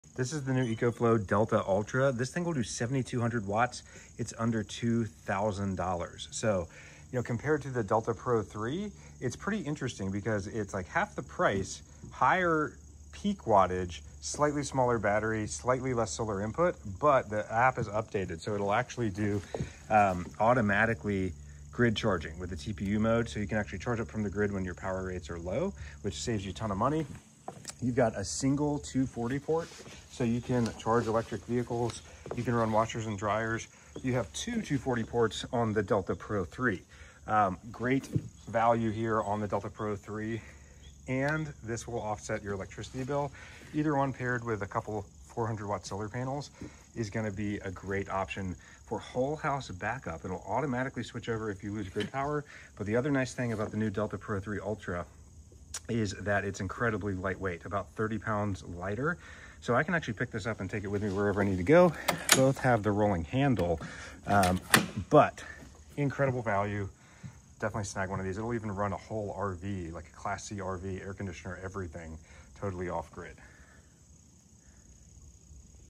Ecoflow Delta 3 Ultra Vs Delta Pro 3
Get them here:
Delta 3 Ultra
Delta Pro 3
Delta Pro 3 Solar Bundle
$2,000 off Delta Pro Bundle
EcoFlow 400 Watt Solar Panel

The EcoFlow Delta pro three ultra is an incredible value at under $2000, with 7200 W of power! Great for a whole house backup, camping RVs or emergency prep. The Delta pro three offers slightly higher capacity and more solar input, but also a higher cost. Both are great options, especially when paired with 1000+ watts of solar panels.